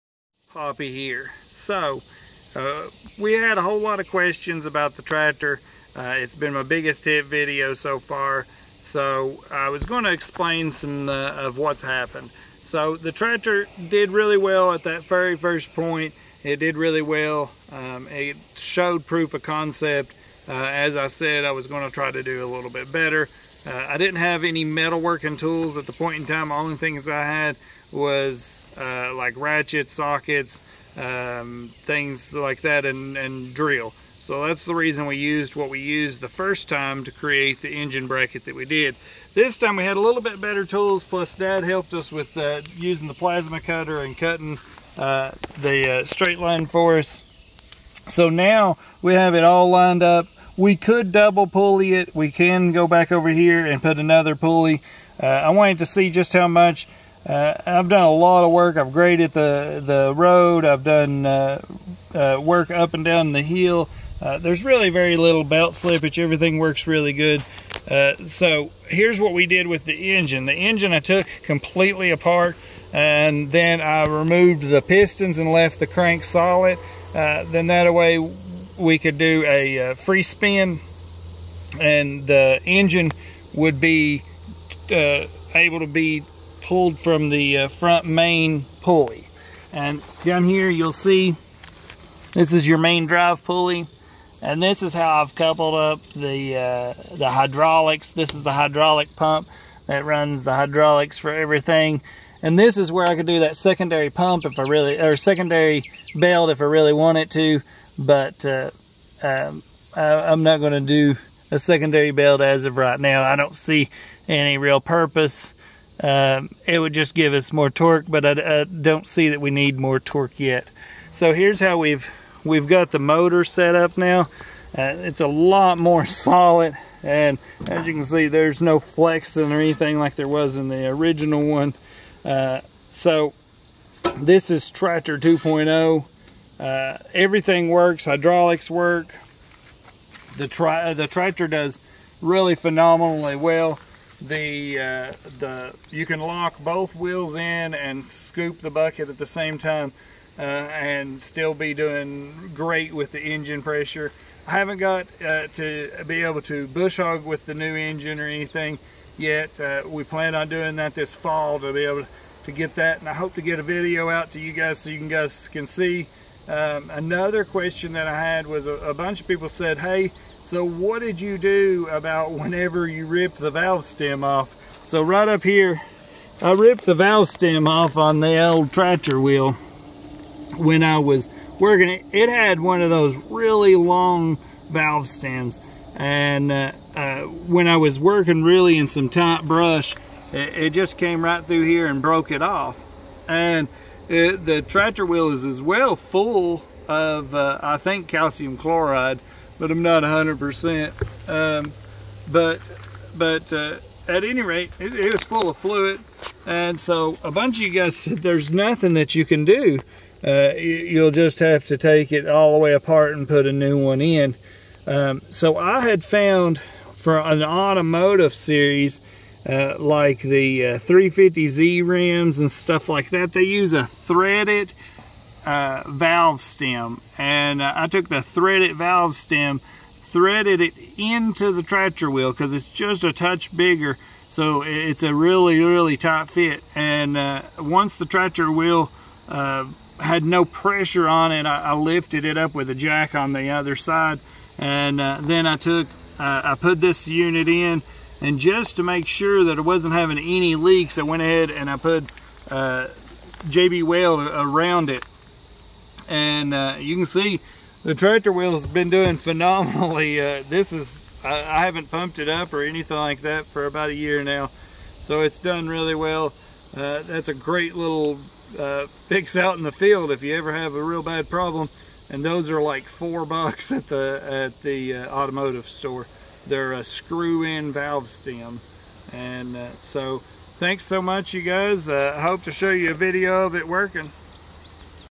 Harbor Freight 420cc engine swap bringing poor people and old tractors together again.
This is the 2.0 version to what we did the first time. We took the pistons off the main crack and use the engines output pulley and put a belt on it and a pulley on the motor and went. The first time we did this it was with no metal tools or anything yet just basic tools like drill and socket set. But this time I had a good solid plate of steel to mount to and the tractor is performing wonderfully. The old tractor functions all as it should this time. High on the old motor swap didnt work belt would slip too much but now all thats been fixed...